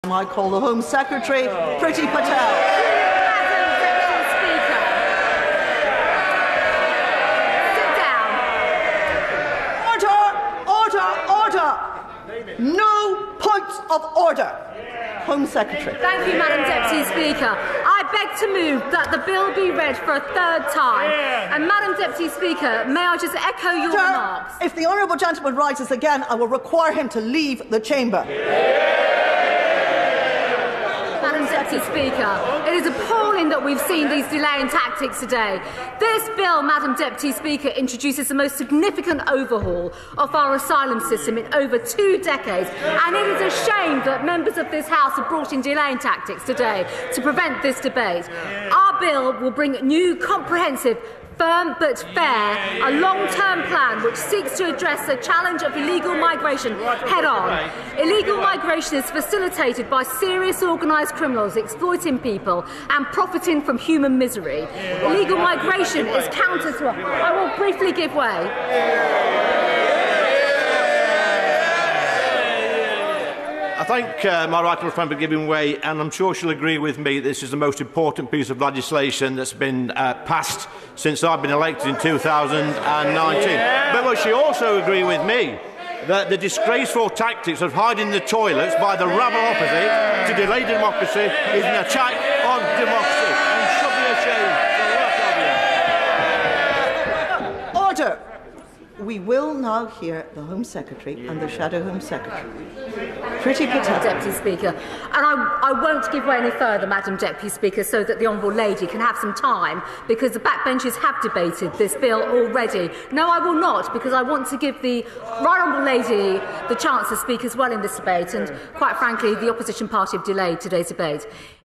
Hiding in the toilets! Raging Tories obliterate "rabble opposition" amid chaos in Commons
The British Government’s flagship immigration legislation has cleared the House of Commons amid chaotic scenes in the Commons.

At one stage Deputy Speaker Eleanor Laing threatened to throw out an SNP MP for repeatedly trying to raise a point of order, while Home Secretary Priti Patel added: “It is appalling that we’ve seen these delaying tactics today.”

Conservative MP Lee Anderson (Ashfield) also said: “The disgraceful tactics of hiding in the toilets by the rabble opposite to delay democracy is an attack on democracy.”

There are toilets in the voting lobbies, with Dame Eleanor at one stage ordering the Serjeant at Arms to clear the voting areas.

The Bill seeks to curb English Channel crossings and change how asylum claims are processed.